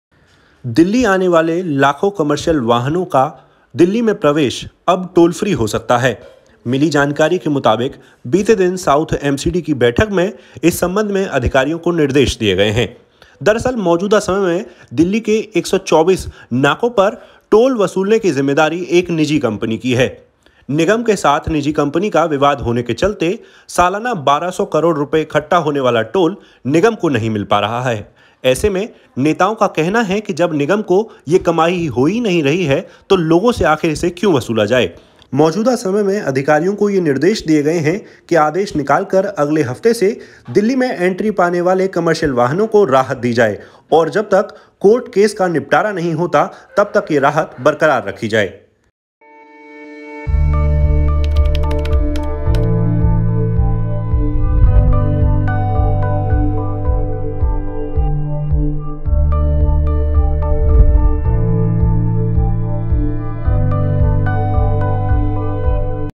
टोल फ्री हो सकती है दिल्ली में गाड़ियों की एंट्री, लाखों को मिलेगा फ़ायदा
दिल्ली में प्रवेश करने वाले वाहनों की एंट्री जल्दी ही टोल फ्री होने जा रही है. निगम और निजी कम्पनी के बीच चल रहे विवाद के बीच ये फैसला लिया गया है.